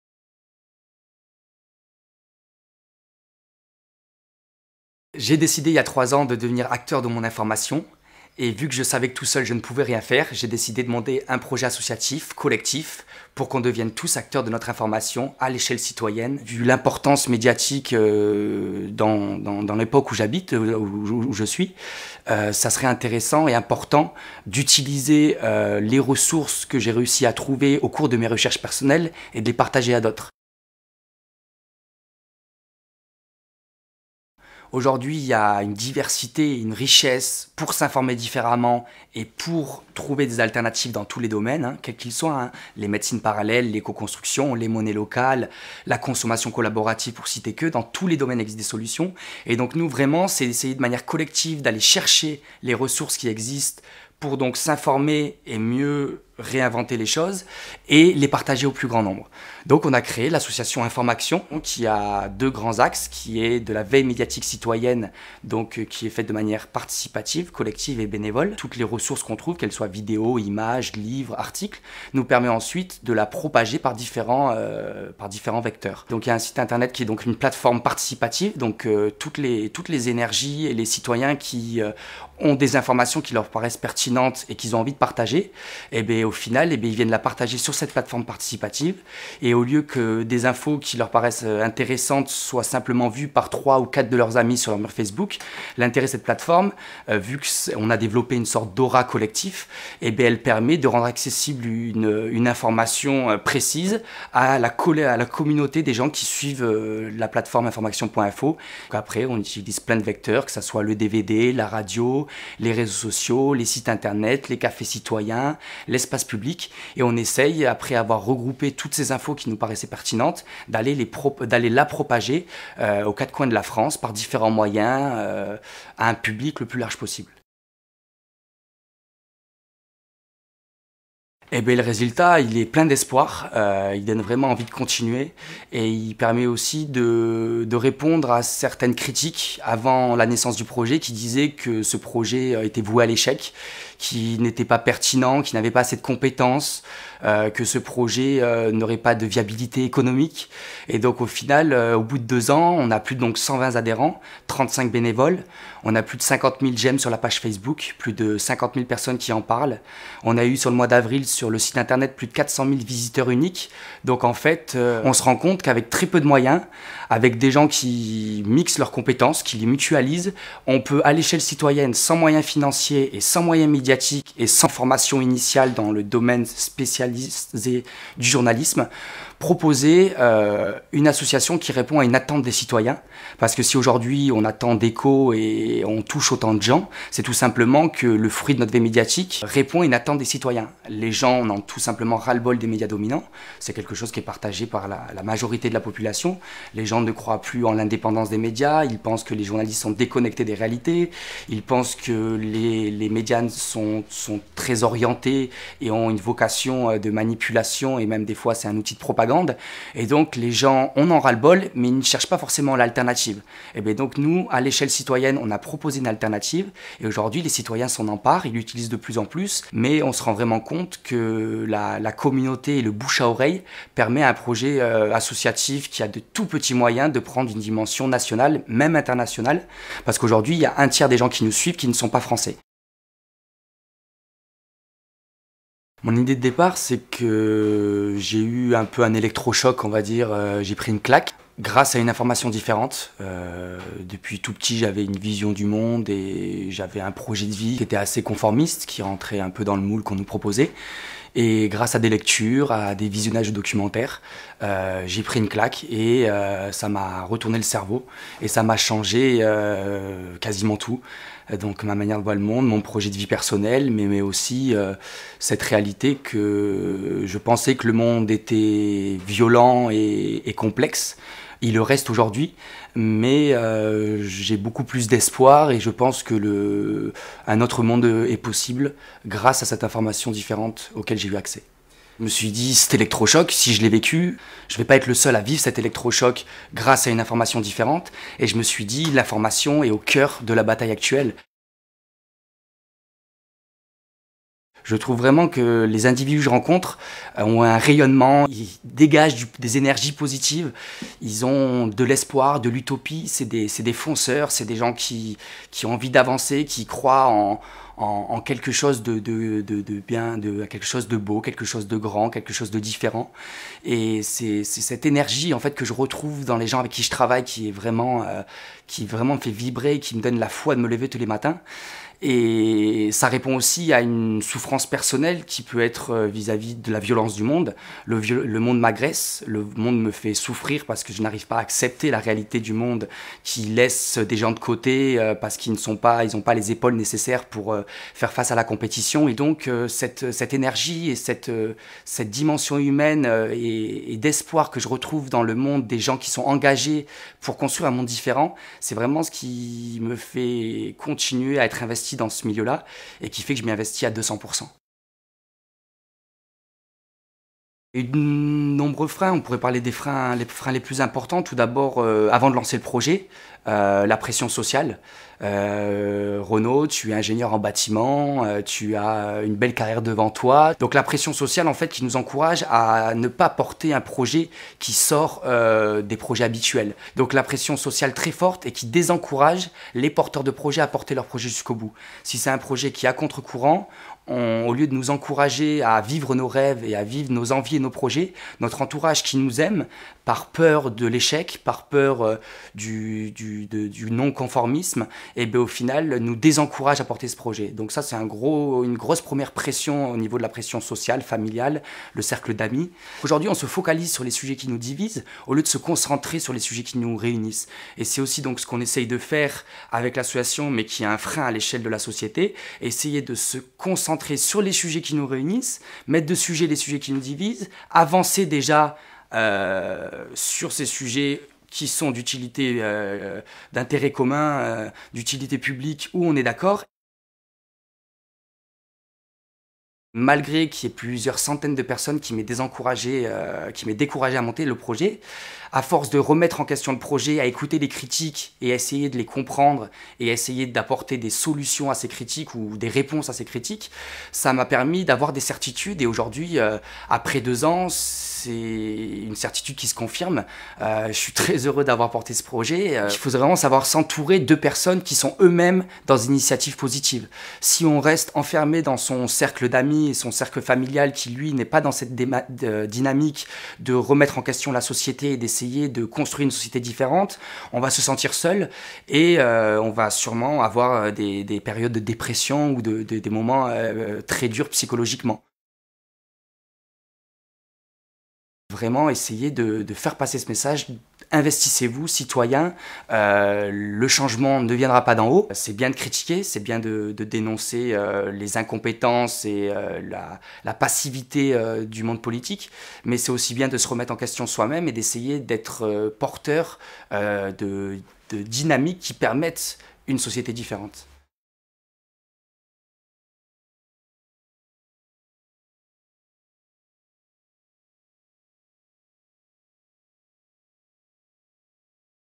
Renaud Schira développe un média citoyen indépendant (version longue)
Renaud a décidé "d'informer autrement".
Pour ça, il a créé Inform'action, un site de journalisme alternatif qui milite pour une information indépendante et citoyenne.
Via son site internet, l'association produit et diffuse des infos parfois négligées par les médias classiques. Le site est équipé d'une plateforme participative. Tout un chacun peut diffuser ses articles et bénéficier du réseau de l'association.
Résultats : des citoyens acteurs de leur information, des infos qui viennent du terrain, 15 000 visiteurs par jour et près de 3 500 articles, vidéos, liens... en ligne !